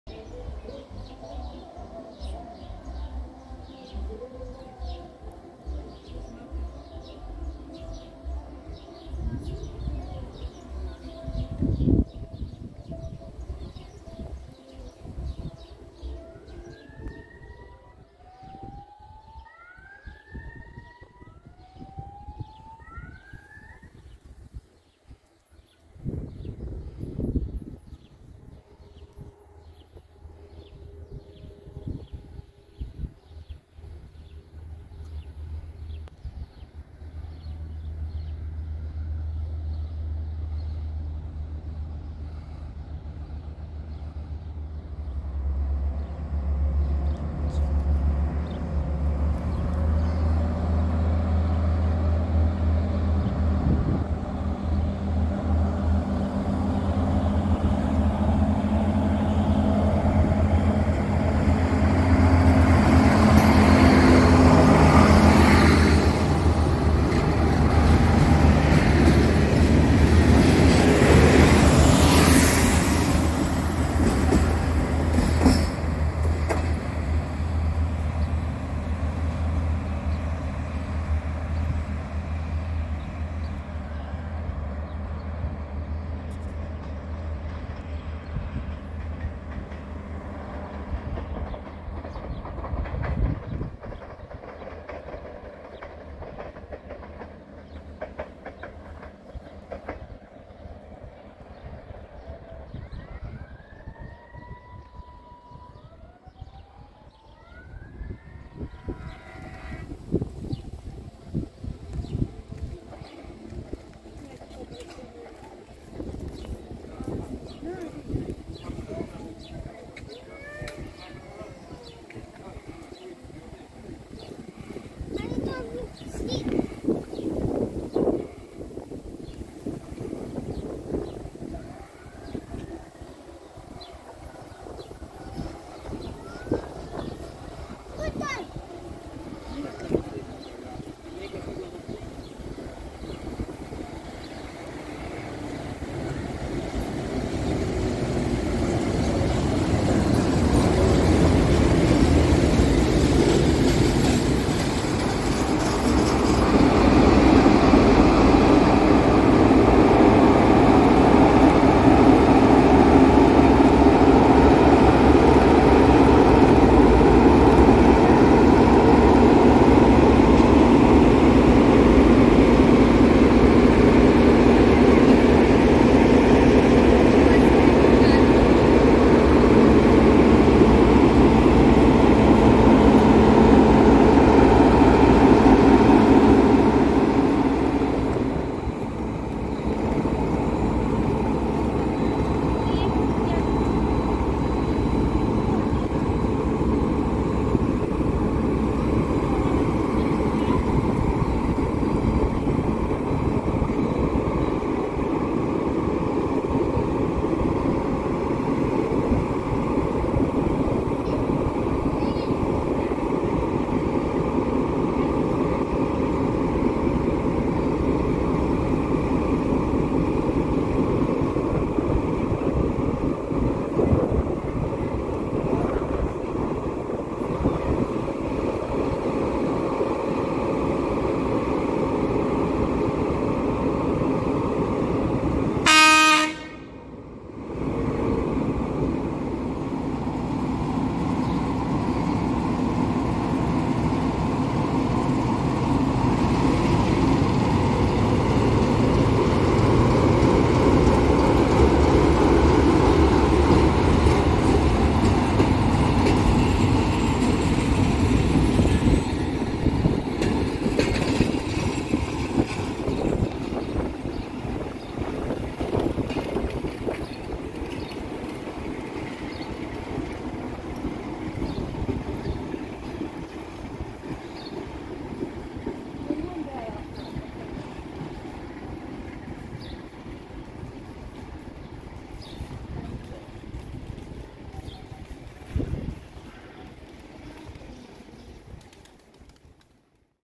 Trains in Scanteia / Trenuri in Scanteia🚆 - 02.06.2023
0:00 - intro
0:18 - R-E 11064 Iasi - Brasov [X 72500 ex SNCF / 5004]
1:50 - R 6313 Tecuci - Iasi [DA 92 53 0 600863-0 RO - SNTFC]